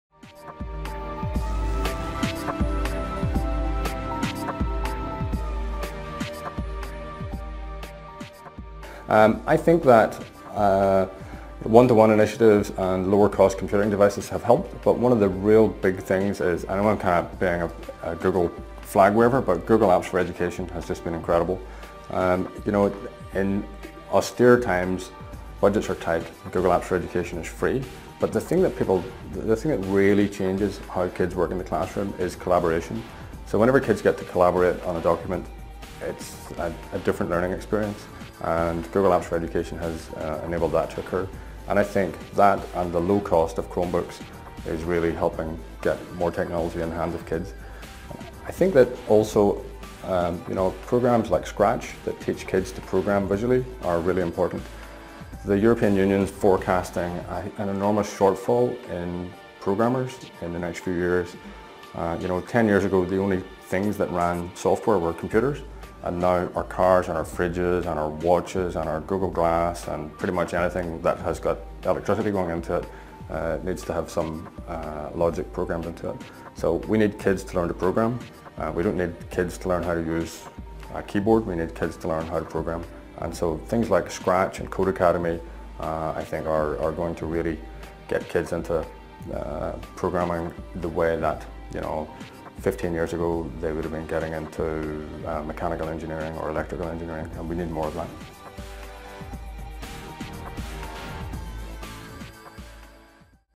Martin McKay - EdTech Game Changers 2015
In this clip Martin discusses the biggest game-changers in Ed Tech.